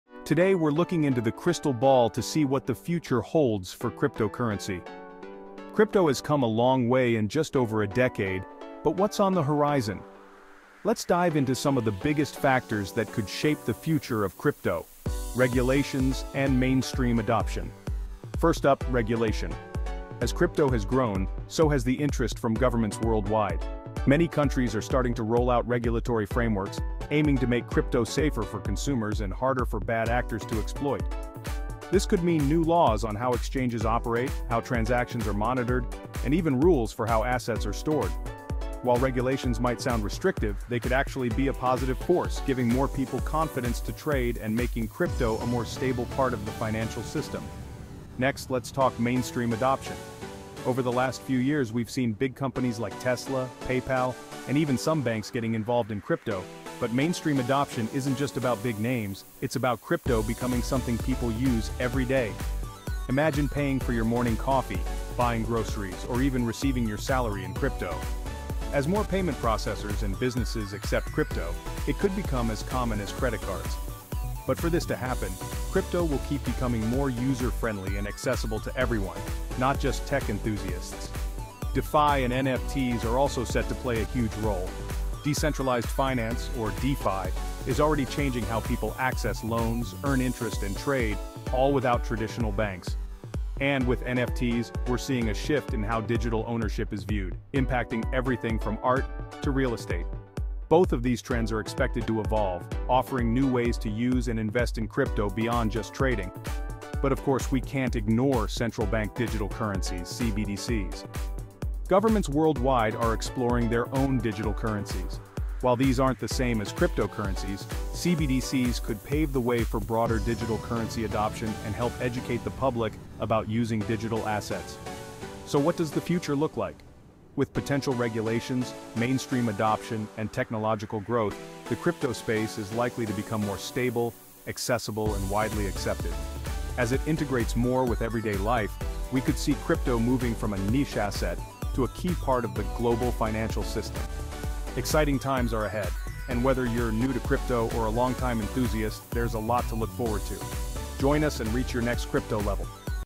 The Future of Cryptocurrency: Regulations, Adoption & More
What does the future hold for cryptocurrency? In this video, we explore the biggest trends shaping crypto’s future—from potential regulations and global adoption to new uses like DeFi, NFTs, and CBDCs. Find out how these changes might impact the way we use and interact with digital currency in daily life.